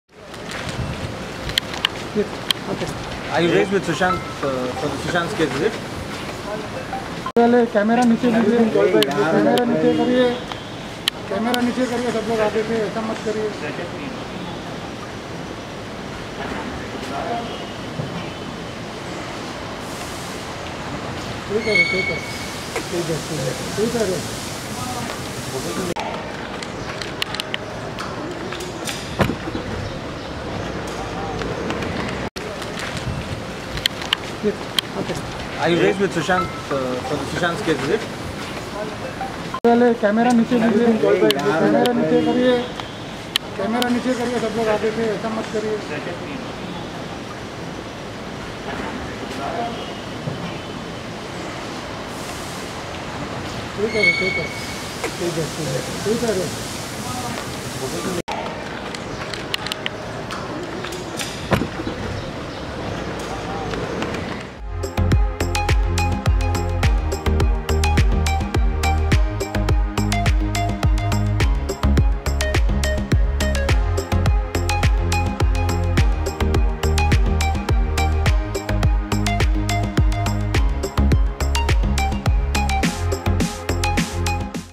Staff Of Sushant Singh Rajput Arrives At ED Office For Interrogation
Sushant Singh Rajput staff Rajat Mewati has arrived at Enforcement Directorate office in Mumbai for interrogation.